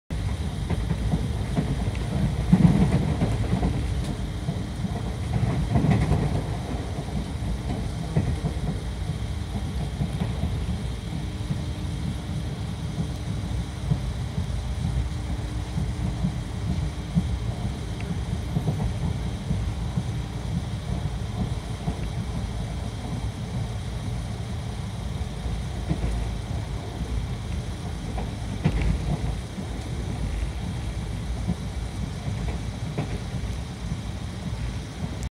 LEGENDARY CSTM - HWH (via Ald) IMPERIAL MAIL BLASTS THROUGH KUMARDUBI
More than 4 hrs 20 mins late running 12322 Mumbai Cst - Howrah Kolkata Superfast Mail (via Allahabad) also called the Legendary Imperial Mail hauled by a Howrah WAP 4 blasts through Kumardubi. Kumardubi is the last station under Jharkhand state and is on the border with West Bengal. West Bengal and jharkhand is seperated by the Kumardubi river. The next station after Kumardubi Barakar is where this train stops.

Howrah - Dhanbad Double Decker Express stops both in Barakar and Kumardubi.

Sorry for this yellow view. This is due to the glass pane.

Camera: Sony Xperia S LT26i